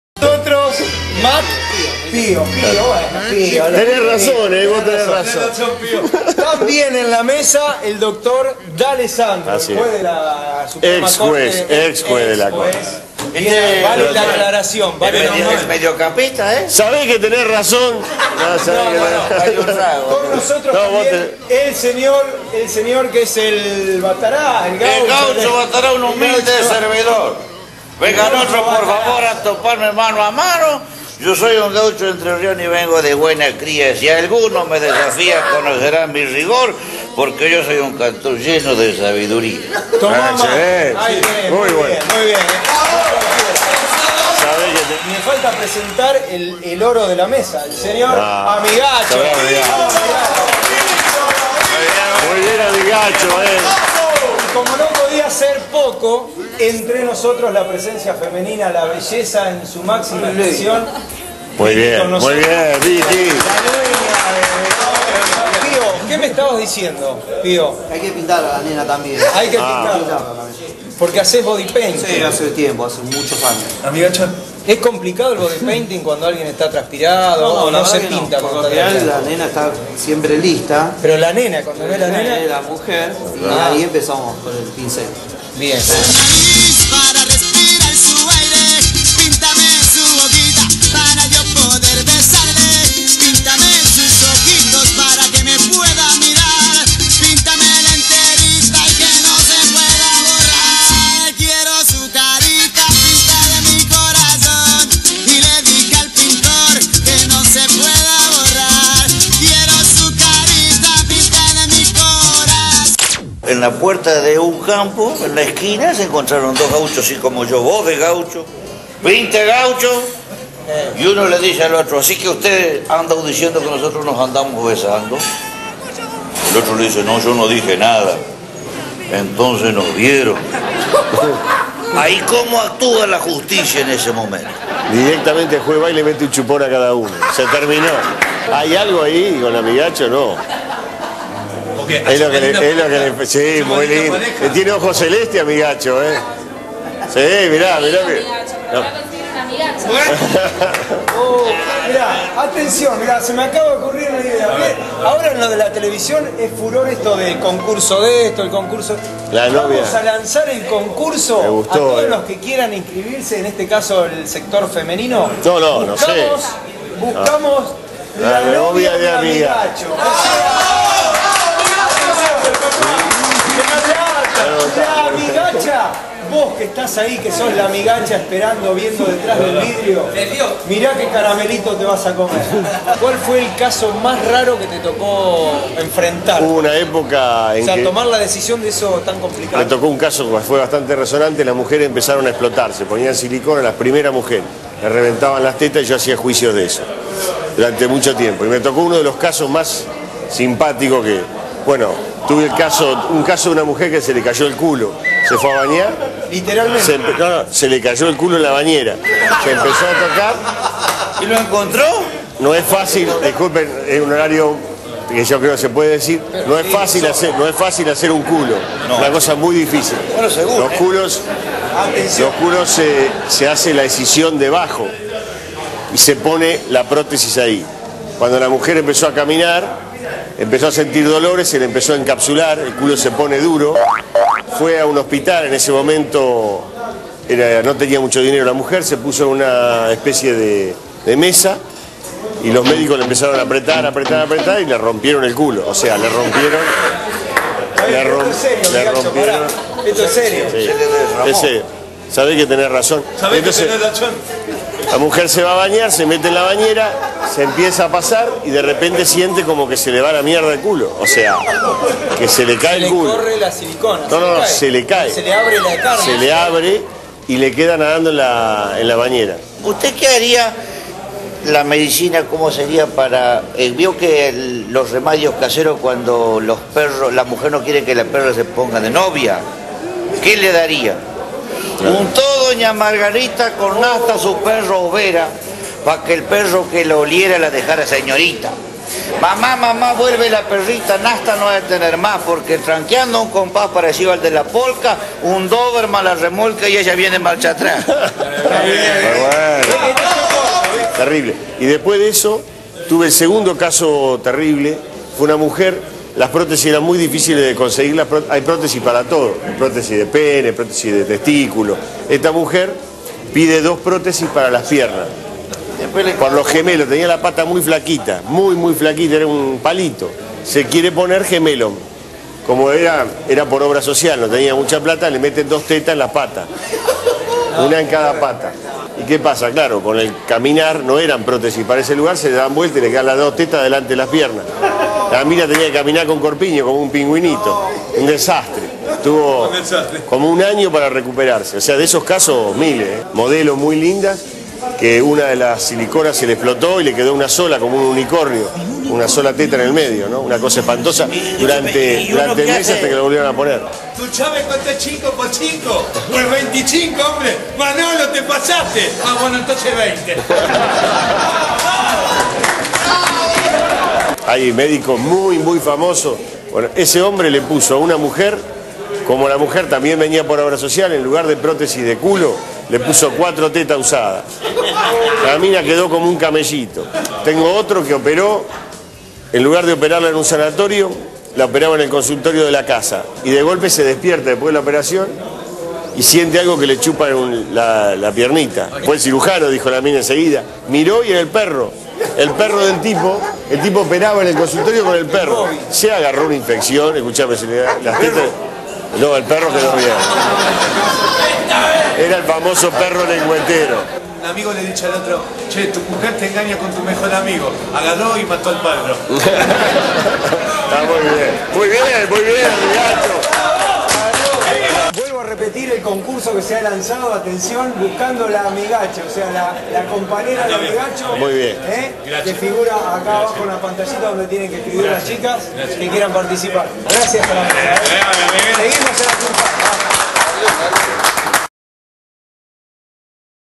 D´alessandro y Fernandito en el café
En esta suerte de Polémica en el bar pudimos ver a Fernandito y al amigable componedor -entre otros personajes- que contó anécdotas sobre operaciones que terminaron en juicios.